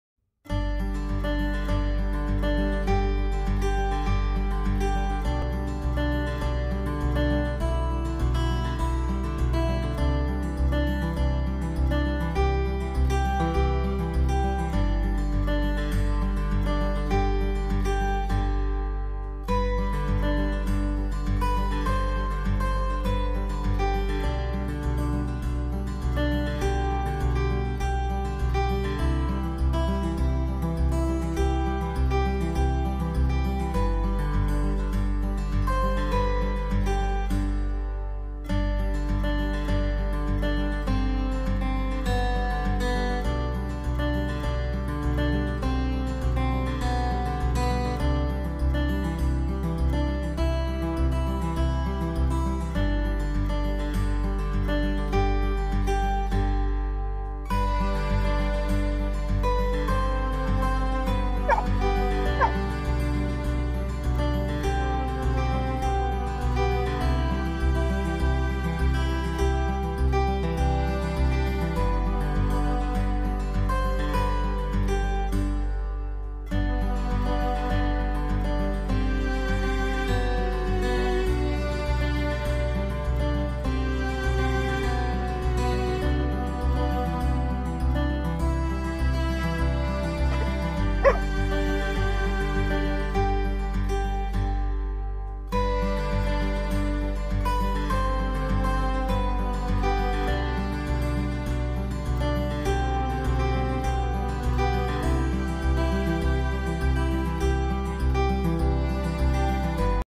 Tricks with Cara the Ridgeback
Just a little video of most of Cara's  tricks that we've worked on. Didn't get to film "play dead" as the ground is waaaay to dirty and cold for a ridgeback to lie down on! Especially in the month November!